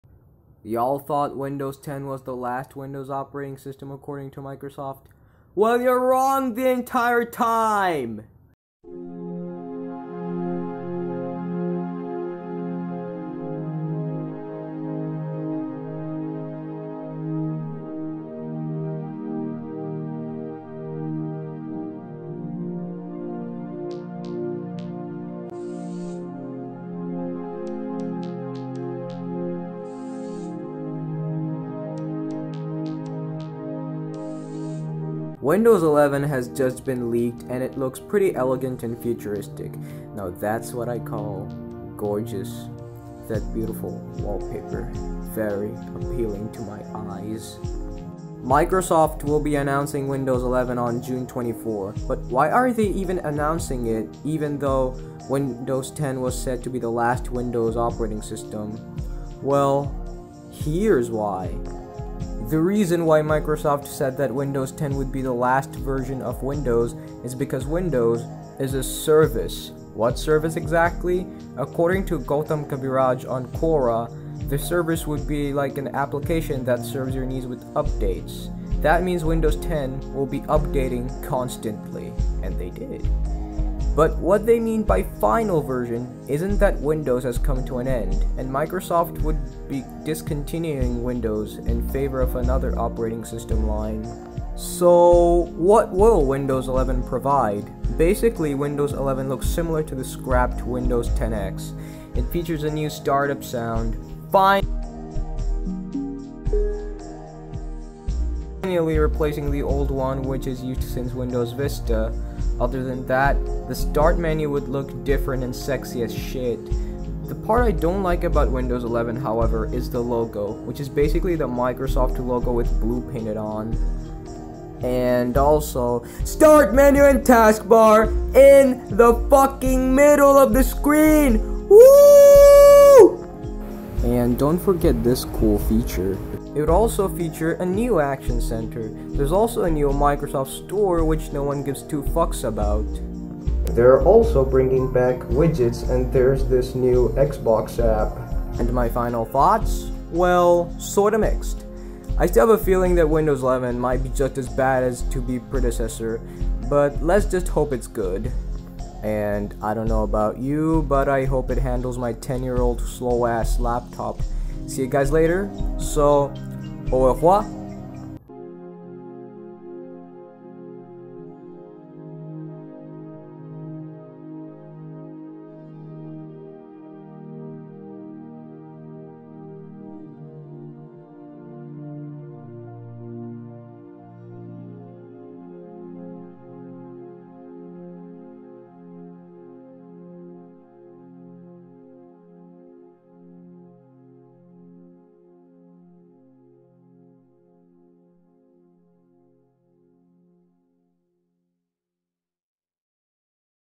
Windows 11 Leaked. What is it like?
Finally, no more Windows 10. We can upgrade to another Windows OS, only if some of our old-ass computers can.